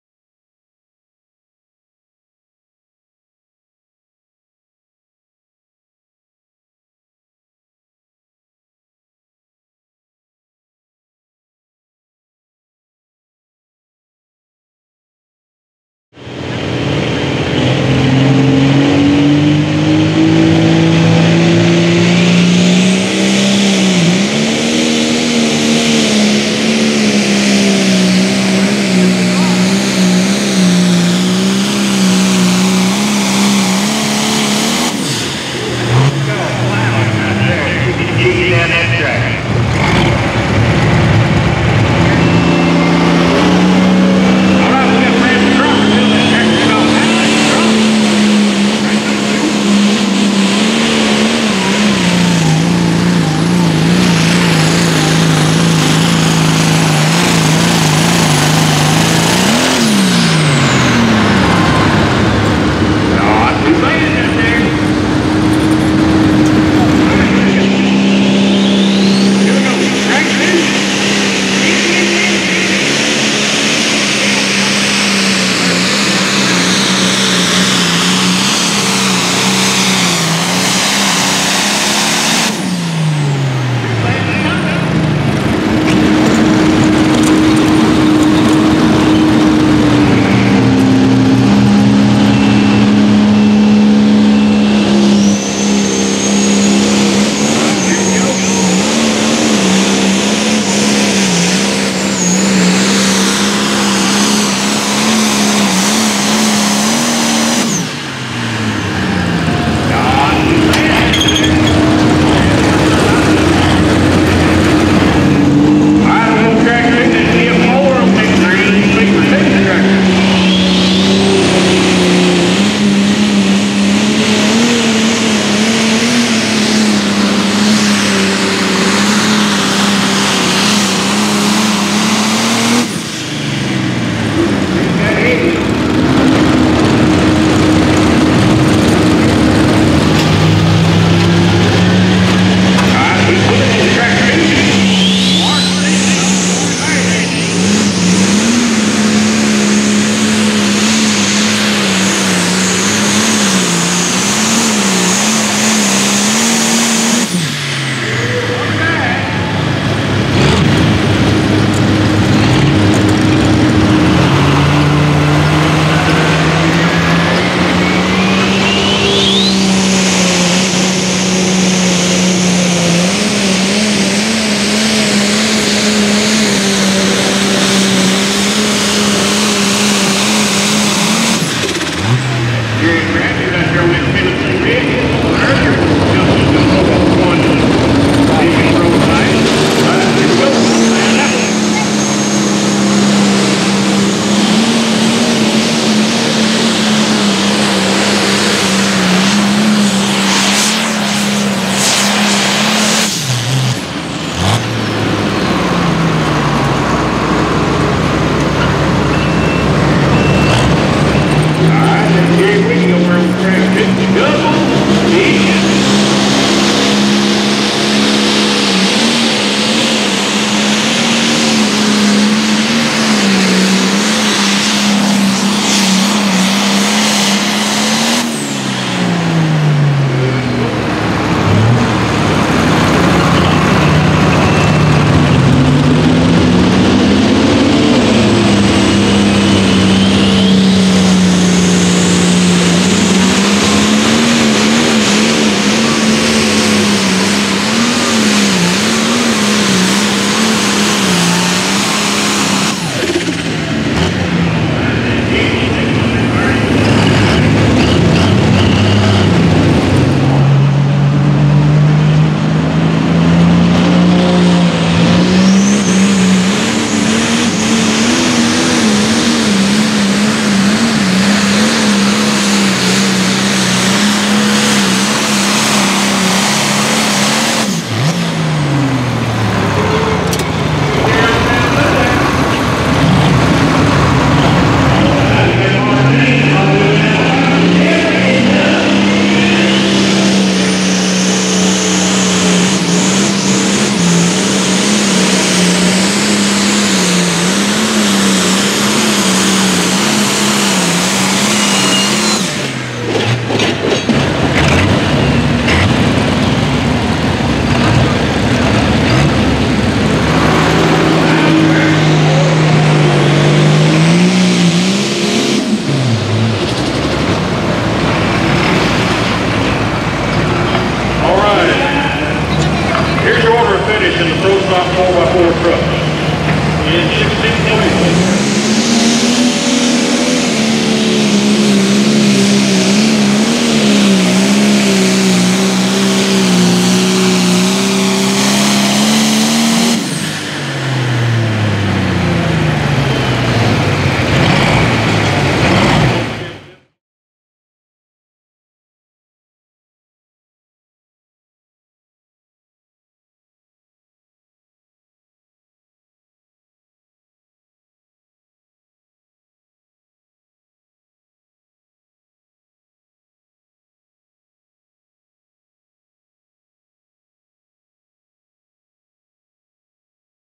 PPL 2019: Limited Pro Stock Tractors | Lawrenceburg, TN | Let’s Go Pulling
Limited Pro Stock Tractors on May 31, 2019 at the Rotary Park in Lawrenceburg, TN. This event sanctioned by the Mid-South Pullers Association, a member state of the Lucas Oil Pro Pulling League (PPL).

Finishing Order:
(1:08) 1. J.T. Calvery - Pickin’ & Ginnin’ - 309.28
(4:20) 2. Jed Pettus - Dixie Deere - 306.11
(3:32) 3. Gary Witty - Double Vision - 299.95
(5:28) 4. Steven Long - Plow Tractor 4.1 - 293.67
(3:09) 5. Bryan Deck - One Bad Apple - 289.63
(0:42) 6. Brandon Cromwell - High Strung - 289.06
(1:31) 7. Nick Bradley - All 4.One - 288.68
(2:20) 8. Larry Dean - Case IH MX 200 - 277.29
(4:46) 9. Mark Dean - Never Enough - 276.31
(3:56) 10. Dennis Dean - Burnin’ The Bucks - 273.15
(2:44) 11. Jared Bradley - Tennessee Red - 272.73
(1:56) 12. Jeff Moore - Green Fever Too - 272.09

Johannes Meuleners
JP Pulling Productions
WWPTV
ASTTQ
Charles Russell
Richard Pruitt
Brock50
TrailerPro